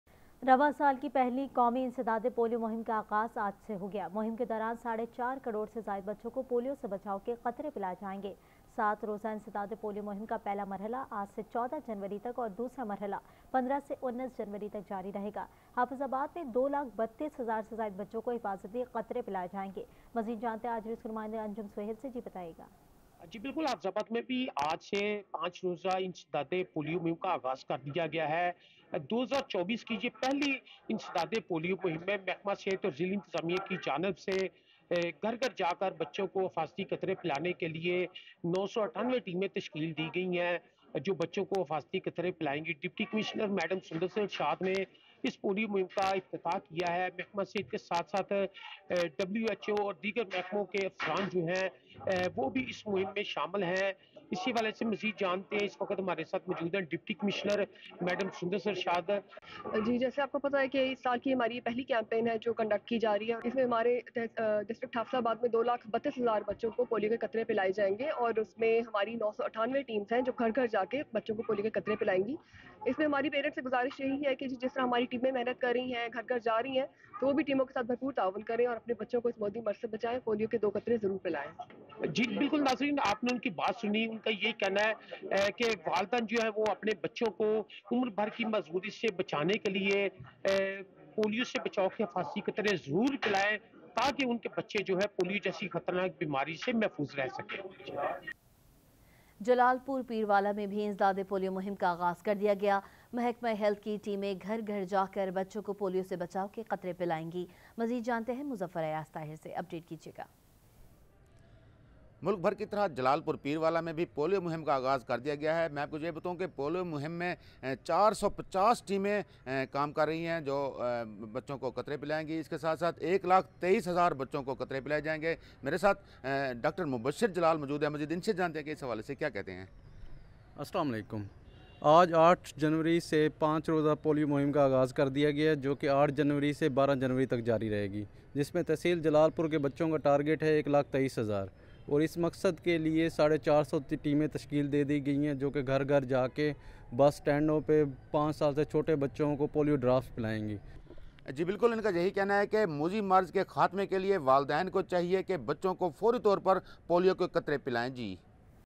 The first national anti-polio campaign of this year has started today - Aaj News
The first national anti-polio campaign of this year has started today - 08 January, 2024 - Aaj News

Spot Light With Munizae Jahangir:
shorturl.at/gwFU6